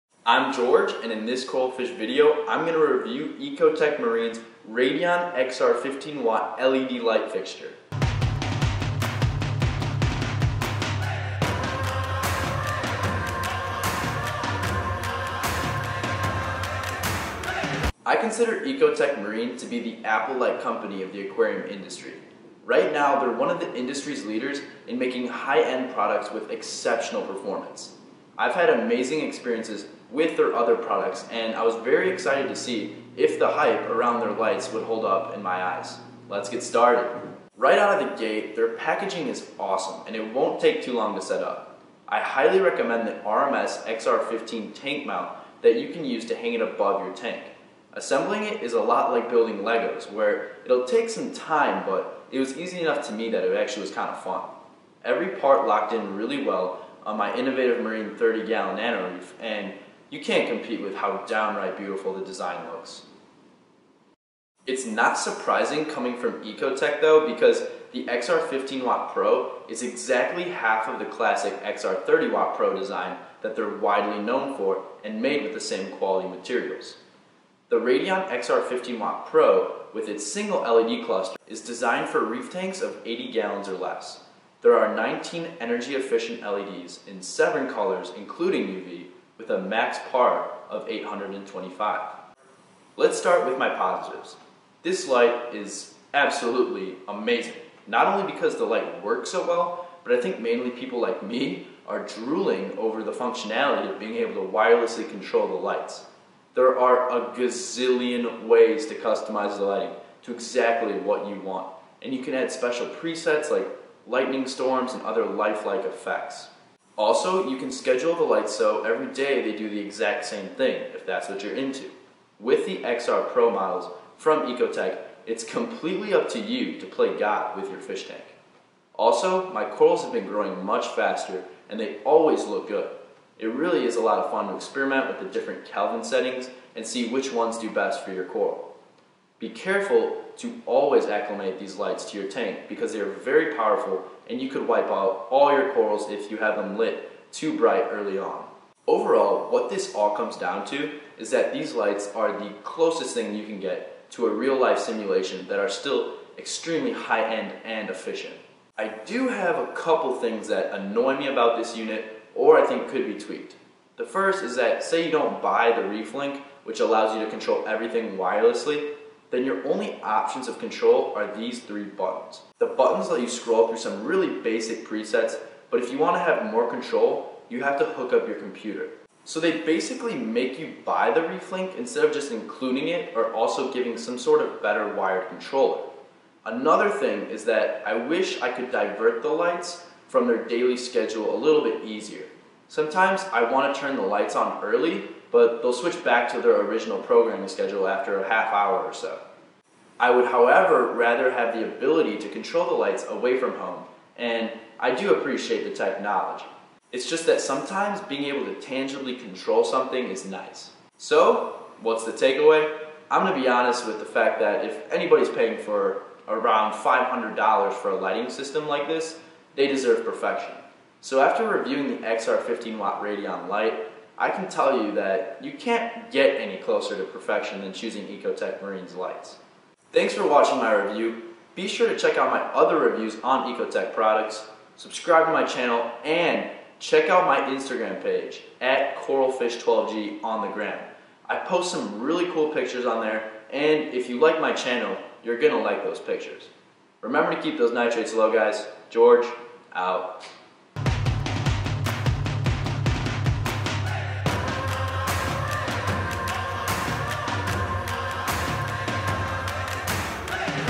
EcoTech Marine Radion XR15w Pro REVIEW
In this CoralFish video I am going to review EcoTech Marine’s Radion XR15w Pro Led Light Fixture. I consider EcoTech Marine to be the Apple like company of the aquarium industry. Right now they are one of the industry’s leaders in making high end products with exceptional performance. I’ve had amazing experiences with their other products and I was very excited to see if the hype around their lights would hold up in my eyes.